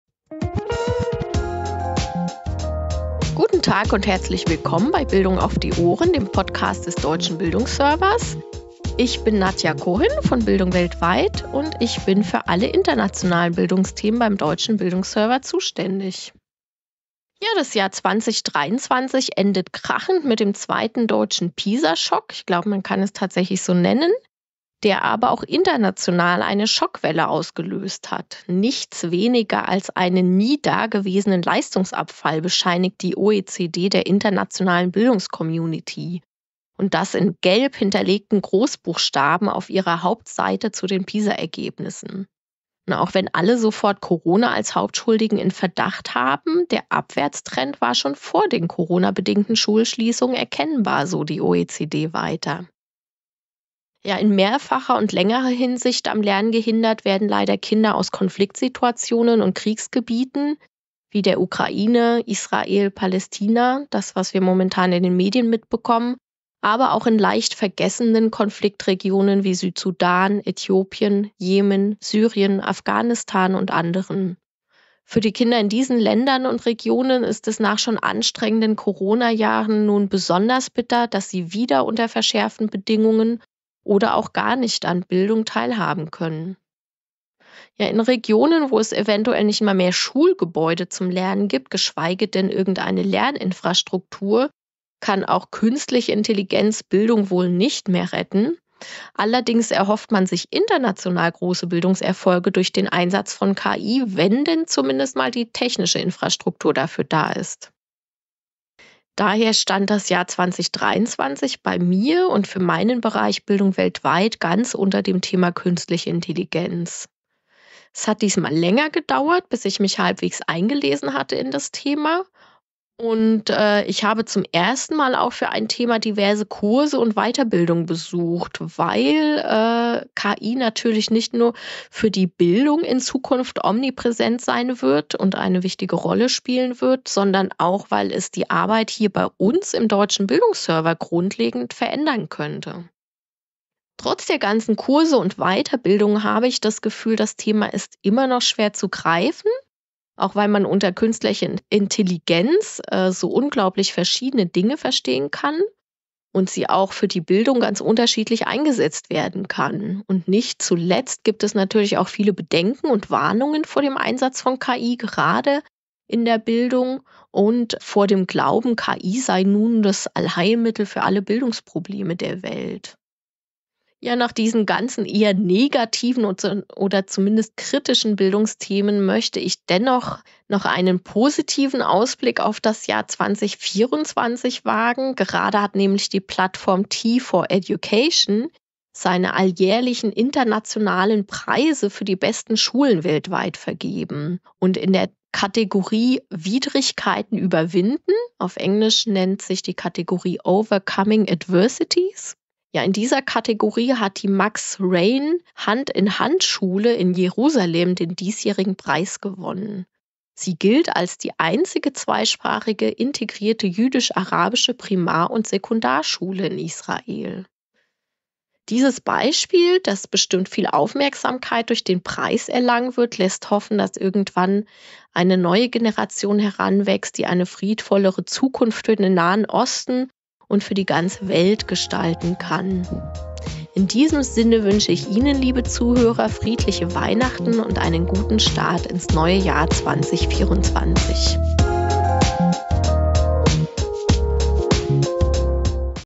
Jahresrückblick 2023 – Bildung weltweit
PISA-Schock, Künstliche Intelligenz und ein internationaler Schulpreis

Podcast zum Jahreswechsel 2023/2024 (5/6)

Die PISA-Studie 2022, die nicht nur für Deutschland, sondern auch international sinkende Schülerleistungen konstatiert, die zunehmende Anzahl von Krisenherden weltweit, in denen sehr viele Kinder noch nicht mal an den regelmäßigen Besuch einer Schule denken können, und die Sorge, Künstliche Intelligenz könnte als Allheilmittel für alle Probleme in der Bildung propagiert werden – das sind die Themen, die Nadia Cohen, verantwortliche Redakteurin für Bildung weltweit, 2023 beschäftigt haben.